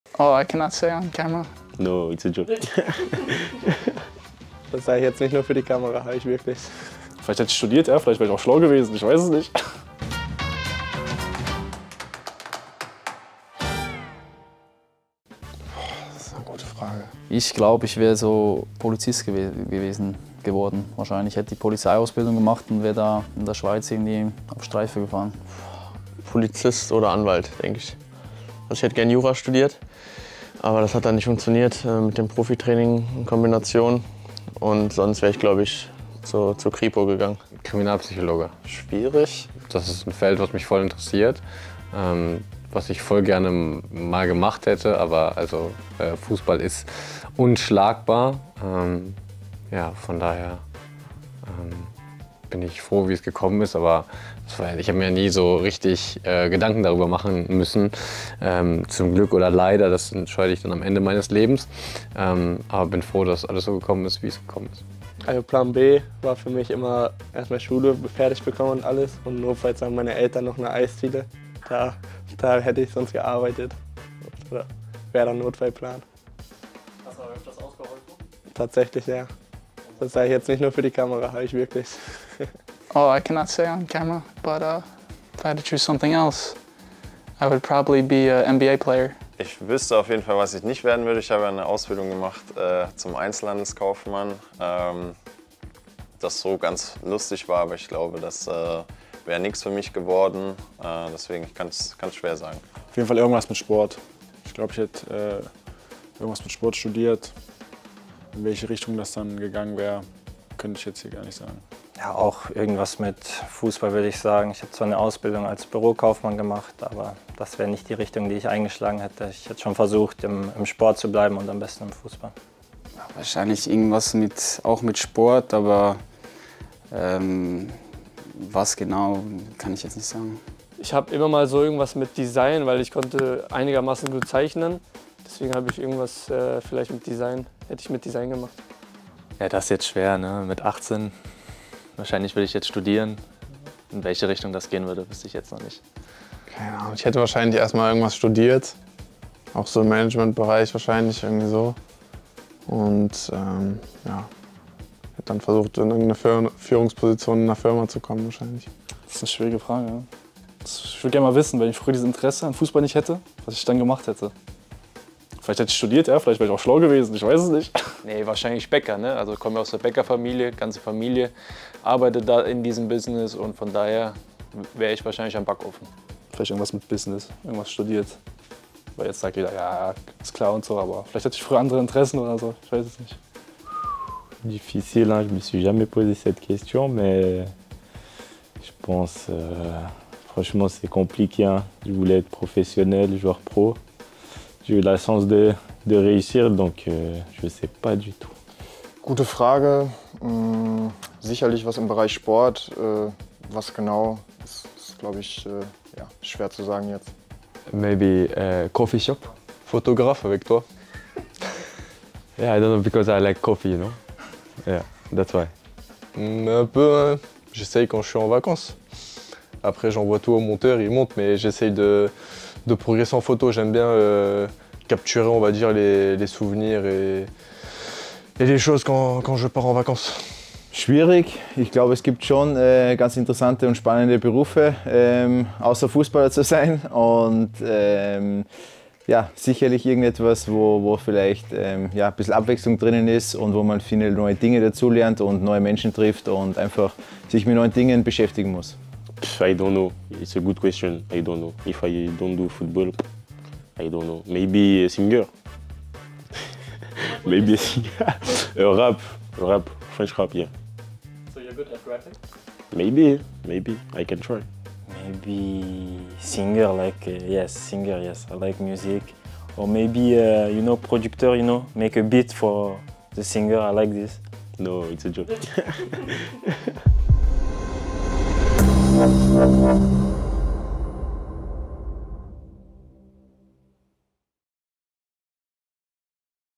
Was würdest du sonst beruflich machen? 💬 Sag mal... | FohlenInsights
Was würden unsere Jungs wohl machen, wenn sie kein Fußballprofi geworden wären? 😄

🐎🎥 Du willst noch mehr exklusiven Inhalt rund um die Fohlen? Dann schau mal bei tv.borussia.de vorbei!